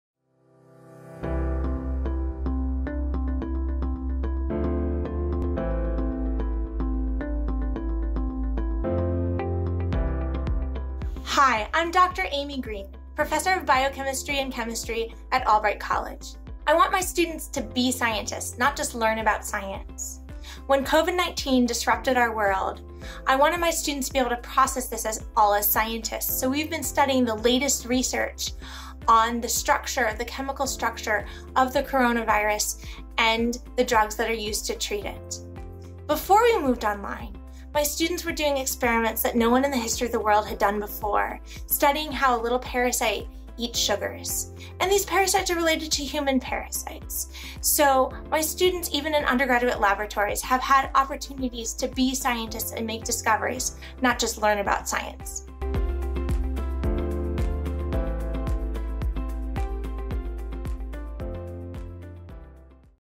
Experience Science at Albright College
Assistant Professor of Chemistry and Biochemistry, Amy S. Greene, Ph.D., explains how her students have opportunities to make discoveries as scientists, instead of just learning about science. No surprise: more than 90% of Albright students who apply to medical school get accepted. Is Albright College, in Reading, Pa., the right fit for you?